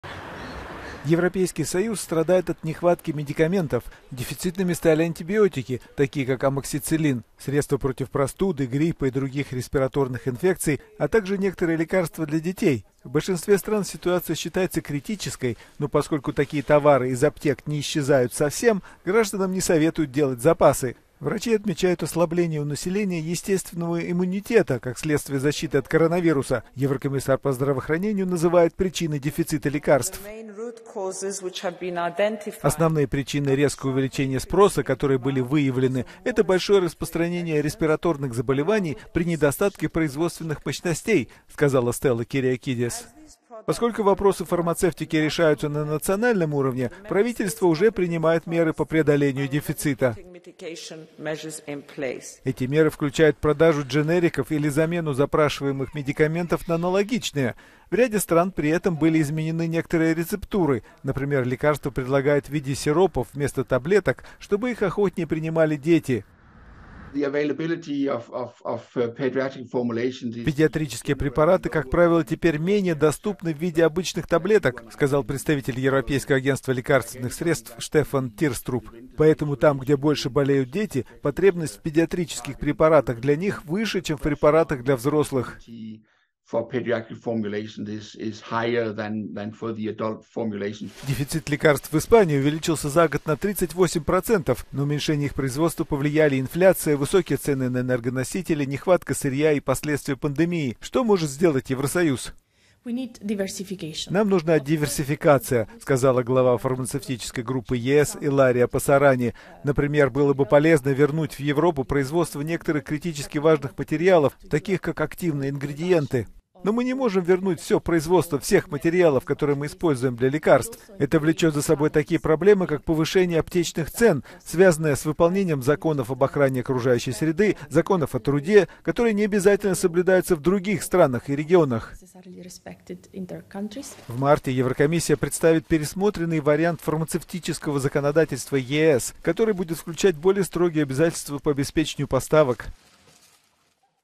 В Евросоюзе не хватает лекарств
Евросоюз страдает от дефицита медикаментов. Еврокомиссия объясняет это распространением  респираторных заболеваний и нехваткой мощностей у фармацевтической отрасли.